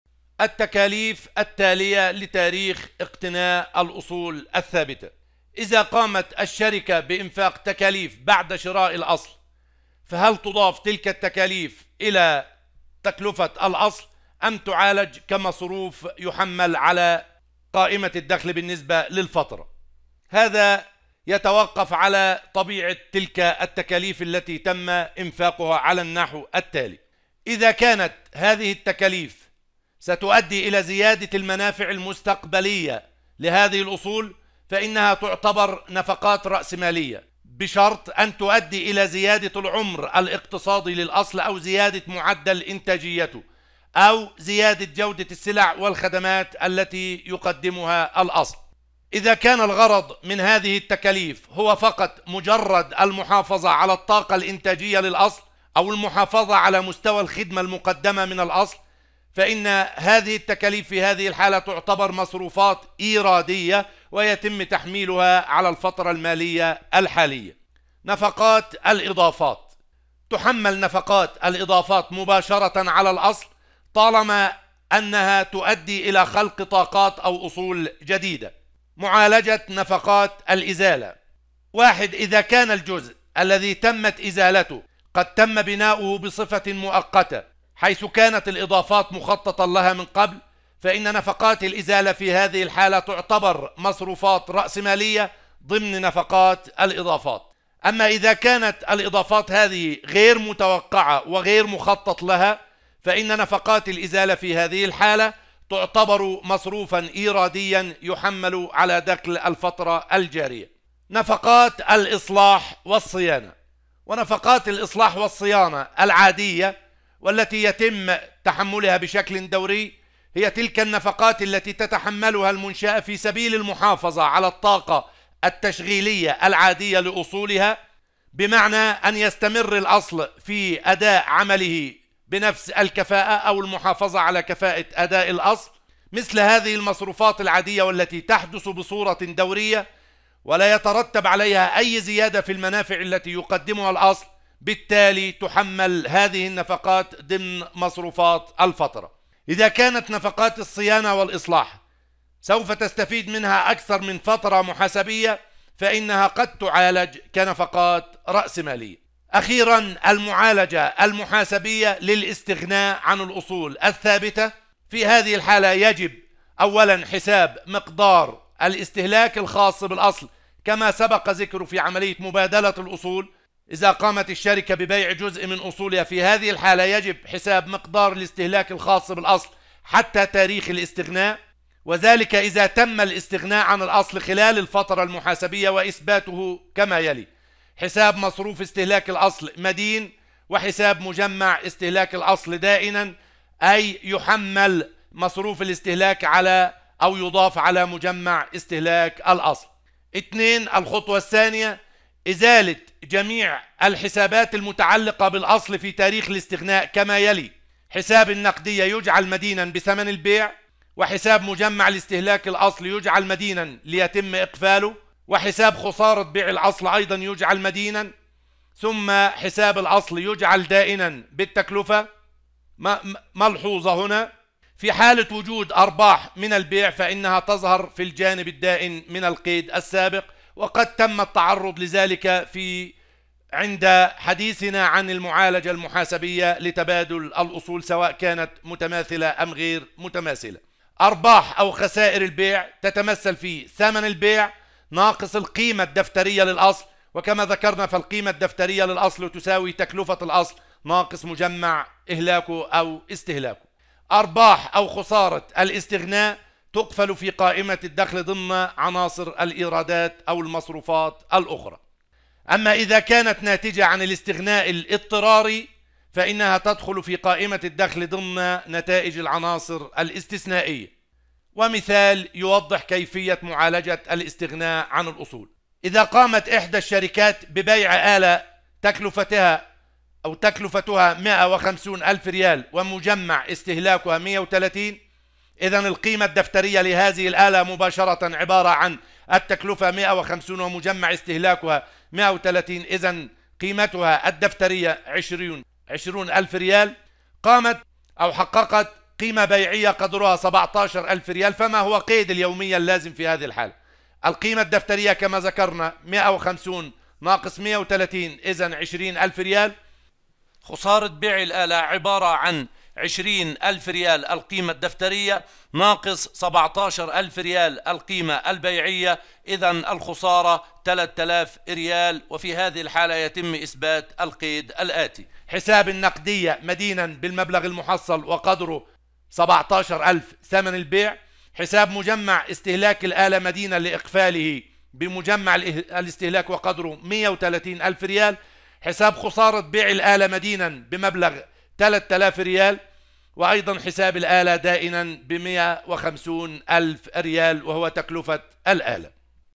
محاسبة المتوسطة 1 - الوحدة 9 : التكاليف التالية لتاريخ إقتناء الاصول الثابتة
- نفقات الاضافات
- نفقات الاصلاح والصيانة
- المعالجة المحاسبية للاستغناء عن الاصول.
التكاليف التالية لتاريخ إقتناء الاصول الثابتة